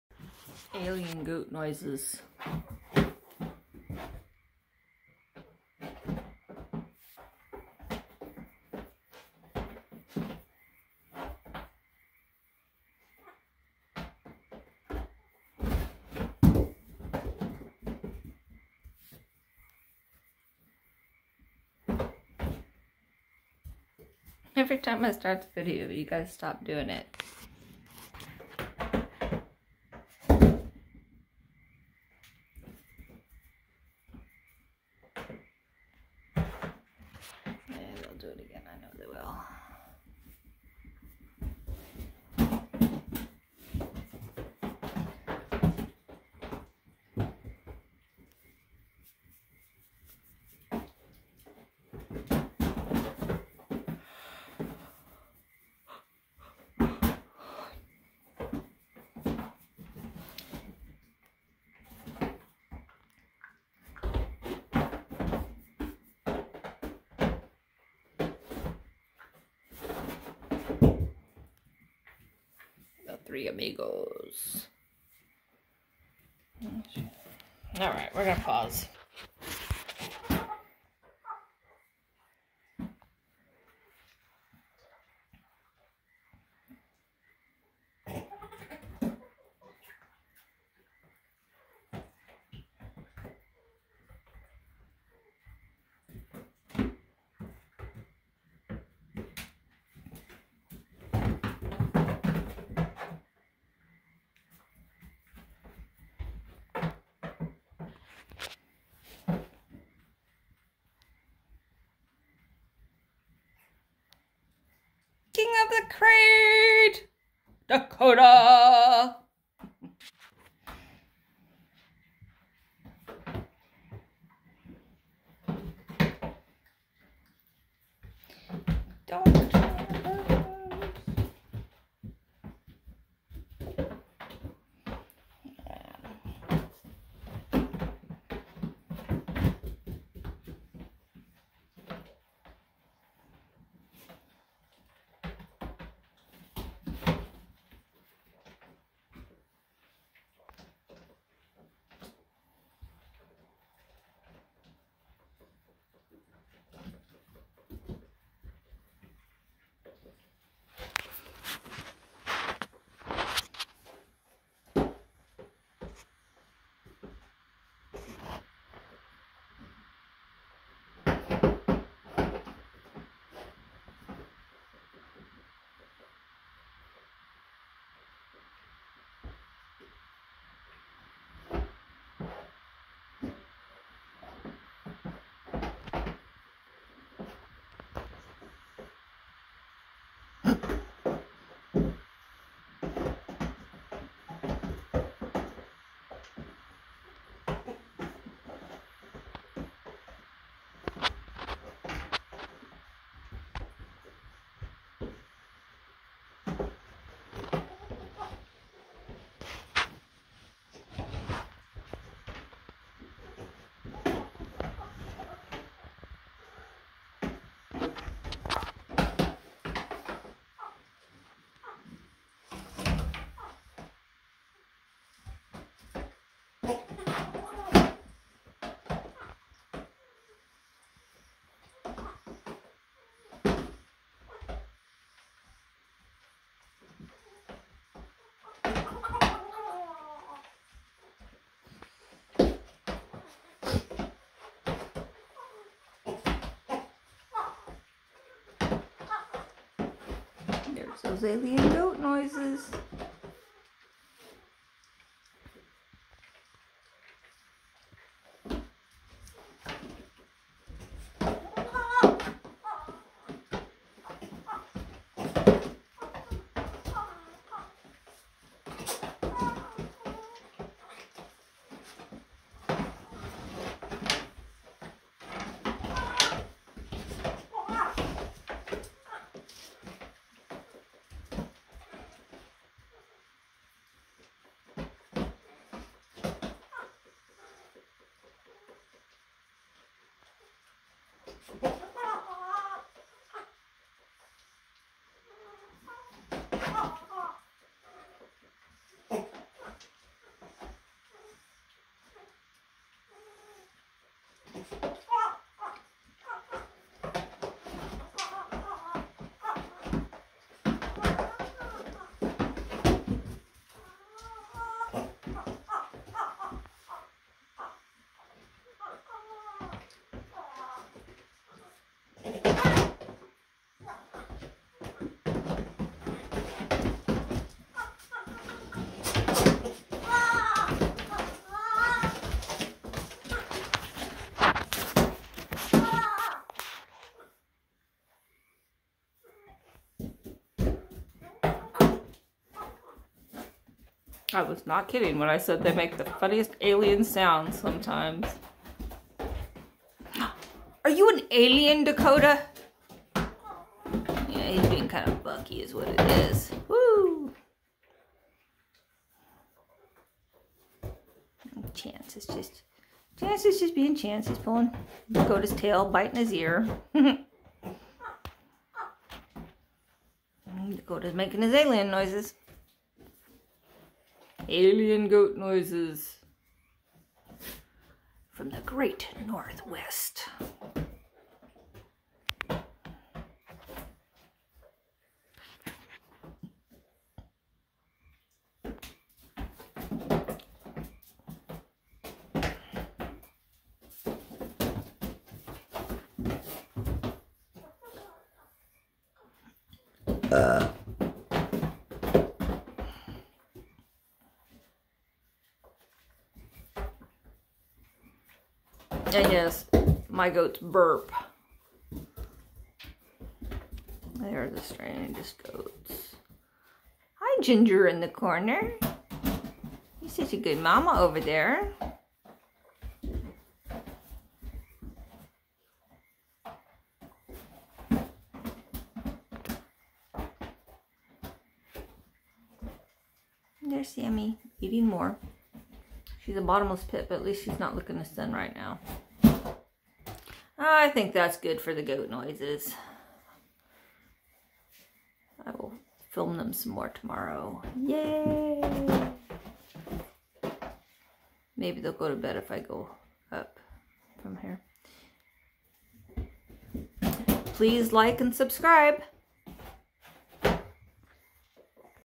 Alien goat noises of the great northwest
Baby goats at play making alien sounds. Just boys being boys really. Lol. Love little baby goats.